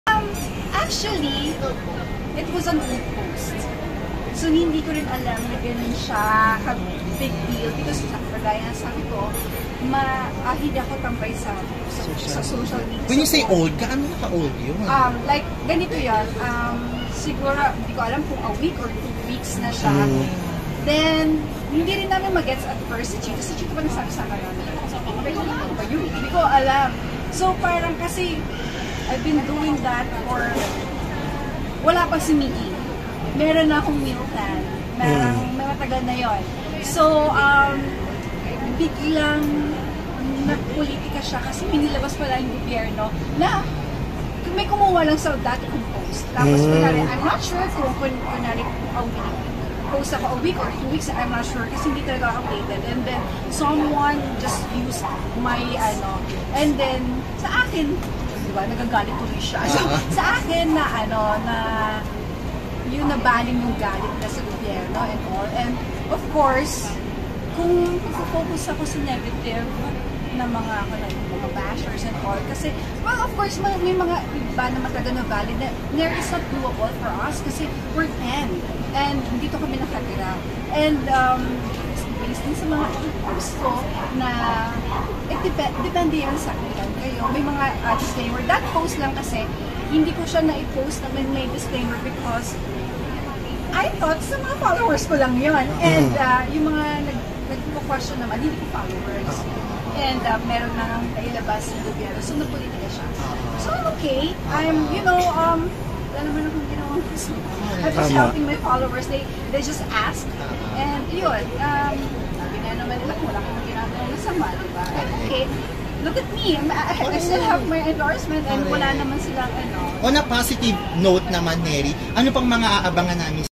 Neri Miranda on the backlash of P1,000 weekly meal budget plan
'I'M JUST HELPING MY FOLLOWERS'

Neri Miranda diniin napulitika ang kanyang post tungkol sa P1,000 budget meal plan. 'Biglang napulitika s'ya kasi may nilabas pala ang gobyerno..' 'Someone just use my (meal plan) and then sakin nabaling na yung galit sa gobyerno.' dagdag pa ni Neri.

Video by Milo Severo